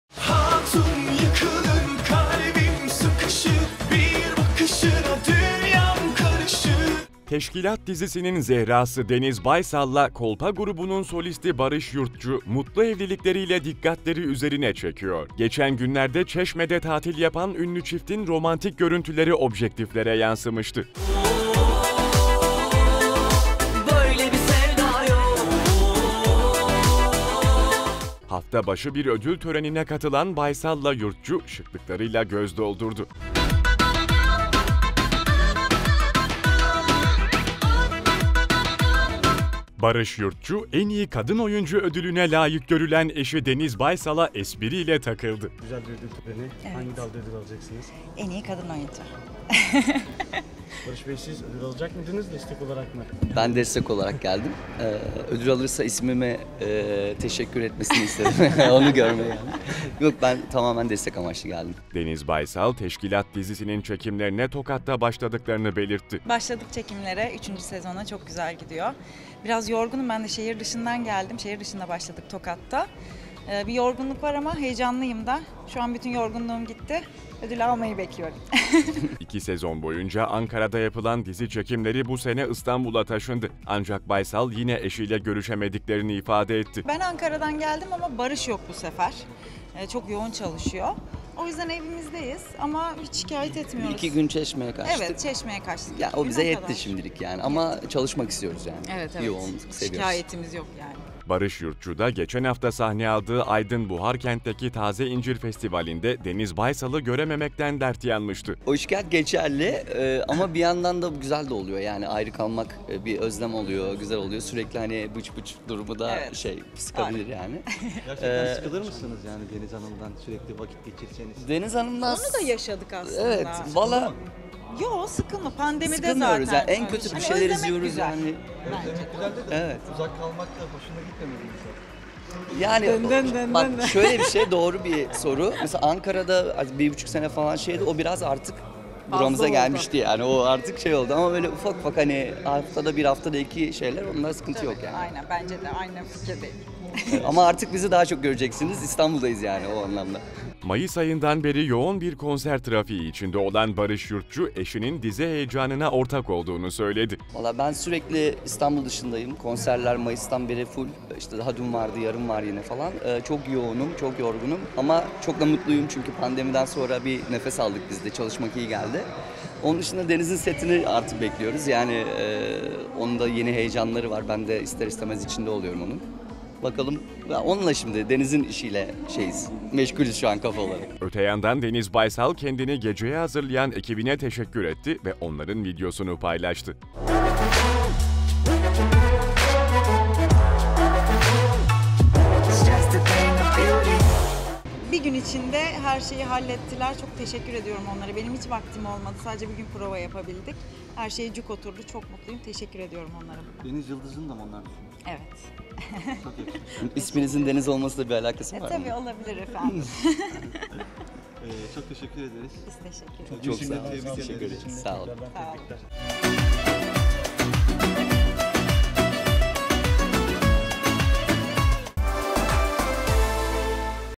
Deniz Baysal ile Barış Yurtçu'nun kıskandıran aşkı
“Teşkilat” dizisinin Zehra’sı Deniz Baysal ile Kolpa grubunun solisti Barış Yurtçu mutlu evlilikleriyle dikkatleri üzerine çekiyor. Geçen günlerde Çeşme’de tatil yapan ünlü çiftin romantik görüntüleri objektiflere yansımıştı.

Hafta başı bir ödül törenine katılan Baysal ile Yurtçu şıklıklarıyla göz doldurdu.

Barış Yurtçu, “En İyi Kadın Oyuncu Ödülü”ne layık görülen eşi Deniz Baysal’a espriyle takıldı.

Deniz Baysal “Teşkilat” dizisinin çekimlerine Tokat’ta başladıklarını belirtti.
İki sezon boyunca Ankara’da yapılan dizi çekimleri bu sene İstanbul’a taşındı. Ancak Baysal yine eşiyle görüşemediklerini ifade etti.

Barış Yurtçu da geçen hafta sahne aldığı Aydın Buharkent’teki Taze İncir Festivali’nde Deniz Baysal’ı görememekten dert yanmıştı.

Mayıs ayından beri yoğun bir konser trafiği içinde olan Barış Yurtçu eşinin dizi heyecanına ortak olduğunu söyledi.

Öte yandan Deniz Baysal kendisini geceye hazırlayan ekibine teşekkür etti ve o anların videosunu paylaştı.

TÜRKSAT 4A
Frekans: 12729
Sembol: 30000
Polarizasyon: V (Dikey)